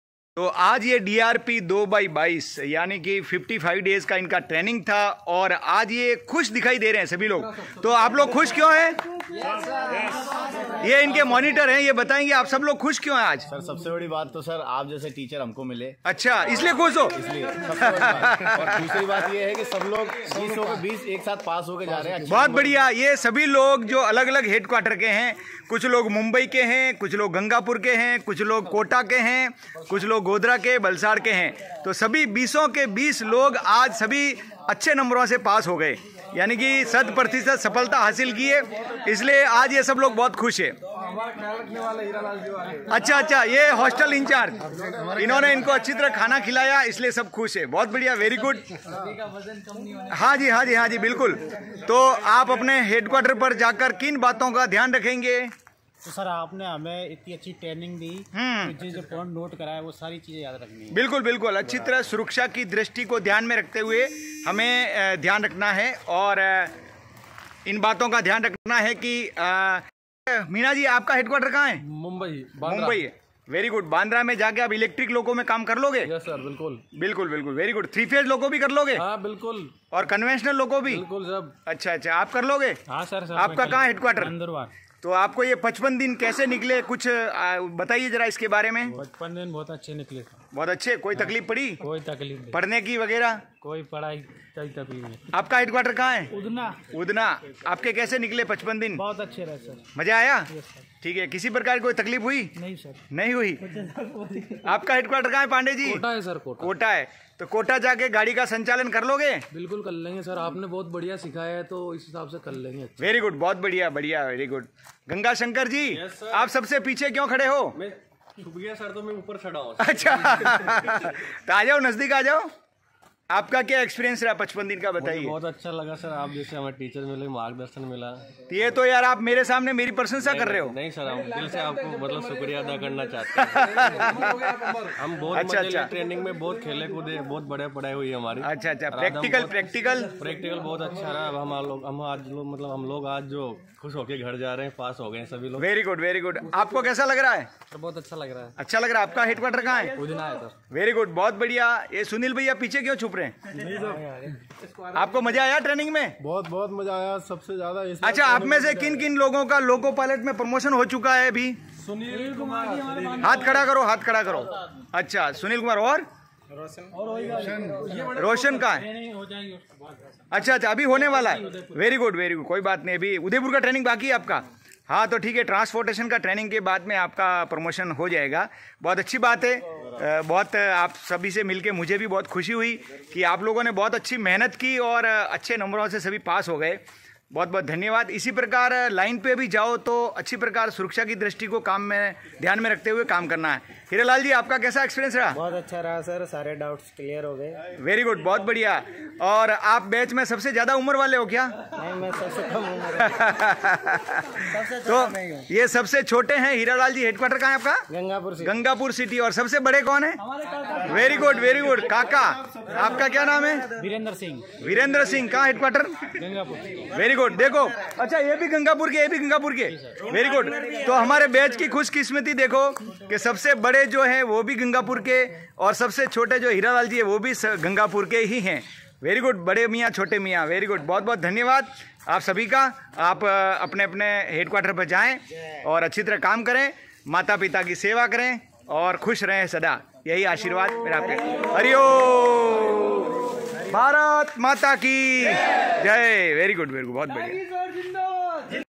DRP 2/22 batch relived from ZETC vadodara. All are so happy.
please follow on khabri app

watch for healthy tips and remedy of disease

from
S L Dangi/vadodara gujarat